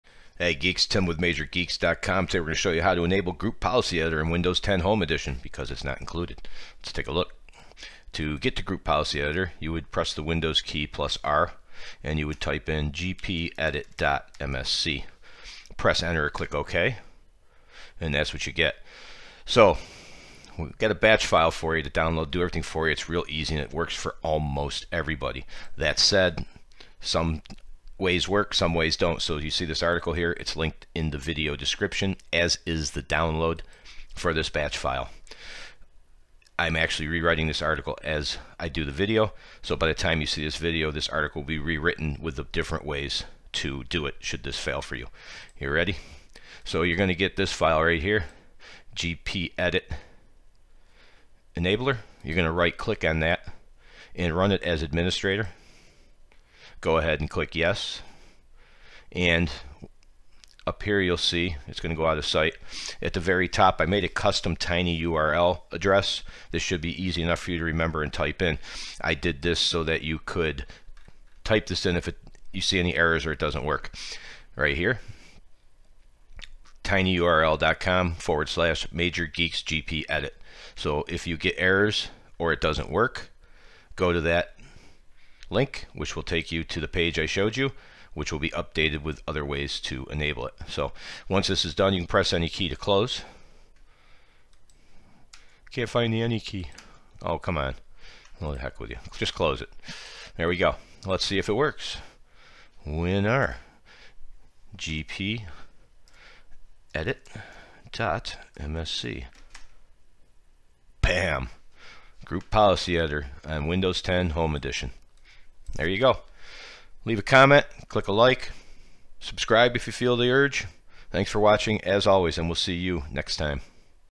Enable Group Policy Editor (Gpedit.msc) in Windows 10 & 11 Home Edition
Enable Group Policy Editor (Gpedit.msc) in Windows 10 Home Edition with PowerShell is a batch file that does all the work for you to add the Group Policy Editor to Windows 10 Home.